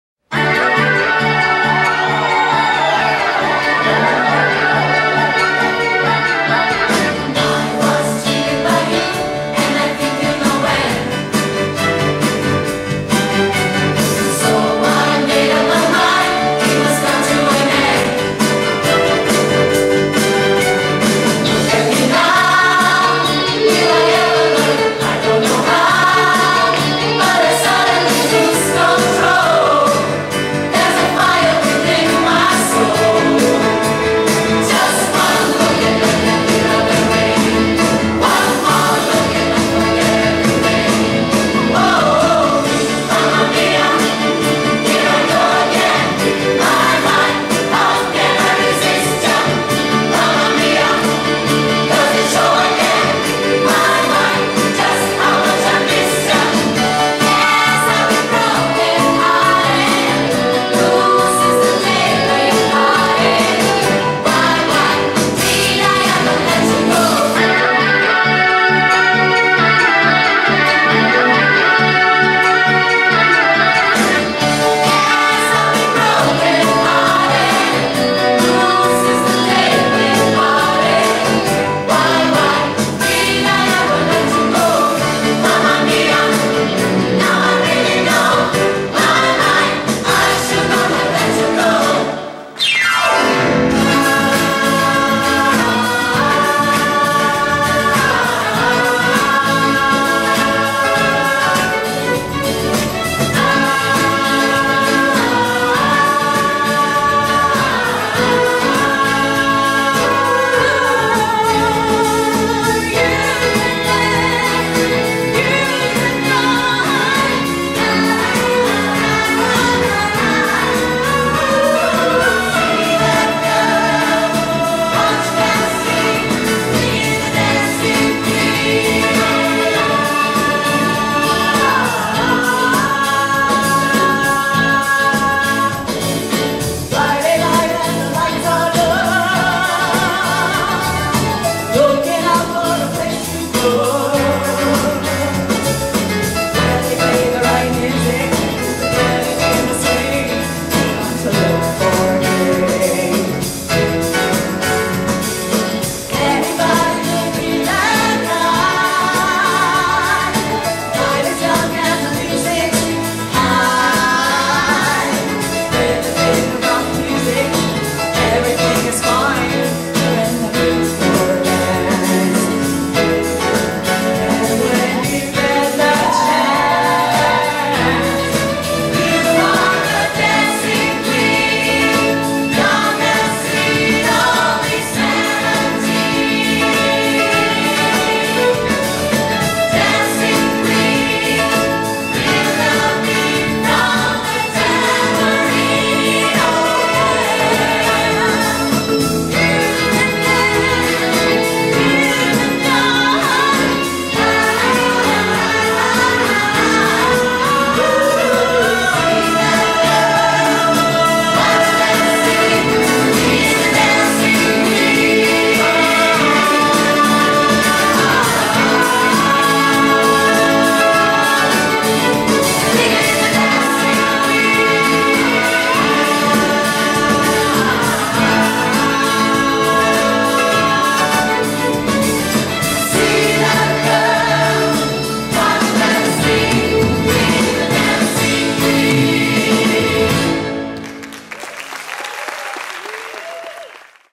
Pré-estréia Mamma Mia! no Teatro Abril
Vídeo com imagens do coquetel de pré-estréia do musical Mamma Mia!, que aconteceu dia 10/11/2010 no Teatro Abril, em São Paulo. Após a apresentação, os atores Cleto Baccic e Pati Amoroso, interpretes de Harry Bright e Sophie, respectivamente, receberam o carinho dos amigos, familiares e colegas de elenco.